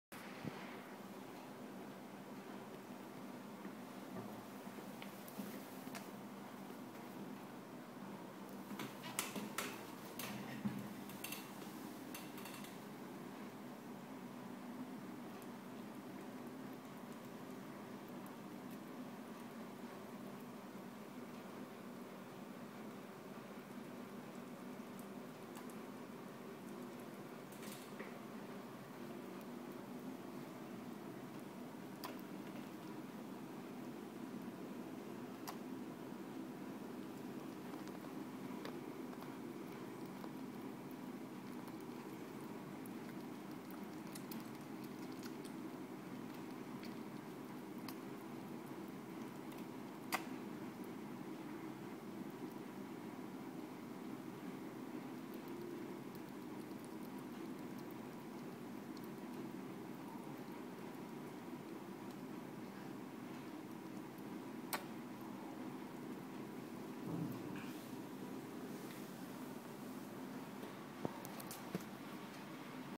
Baby squirrel drinking Esbiliac milk part 2
Enjoying milk before sleep.